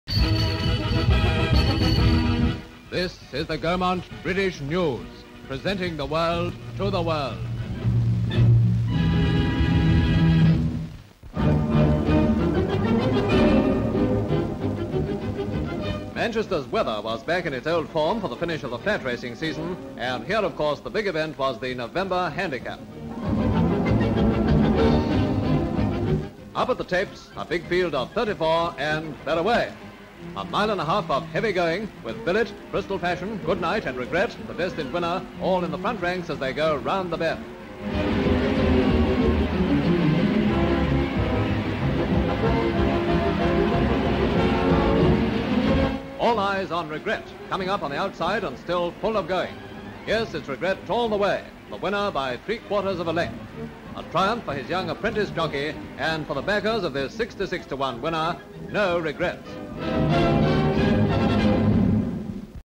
HORSE RACING: Regret wins Manchester November Handicap (1947)
66-1 outsider Regret wins the Manchester November Handicap Steeplechase.

SLATE INFORMATION: Manchester November Handicap

ENGLAND: Manchester:
Crowd in stands in the rain
Horses being led on to racecourse
Spectators watching in grandstand
Horses and jockeys ready at start as race begins
Horses round course
Regret wins race
Apprentice jockey on Regret

Sport; Horseracing
Background: 66-1 outsider Regret wins the Manchester November Handicap Steeplechase.